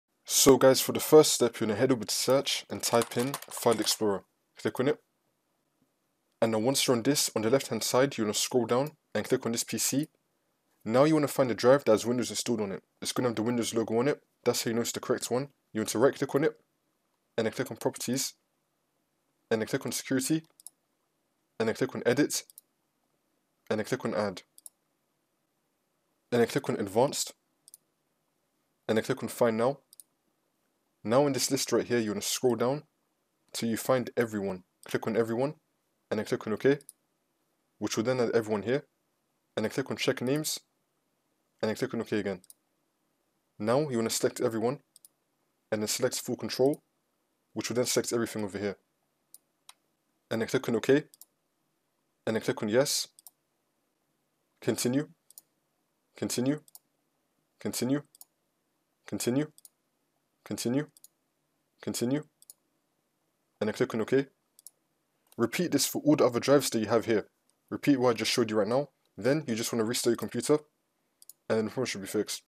How to Fix Folder Access Denied in Windows
How to Fix “Folder Access Denied” You Need Permission to Perform This Action.

The way Windows works is that if a folder, program or a file is being used by another process, then the user will not be able to make any changes to it. If you are getting the error “You need permission to perform this action” while  trying to  delete or move the file/folder, then it is most likely caused by a permissions issue or that file/folder is being used by another program. For example: The folder or file within the folder is being backed up or is being scanned by your AntiVirus program. If the permissions are changed, then you will still be presented with this error – even if you are the administrator. In this guide, i will walk you through a couple of methods to address this issue.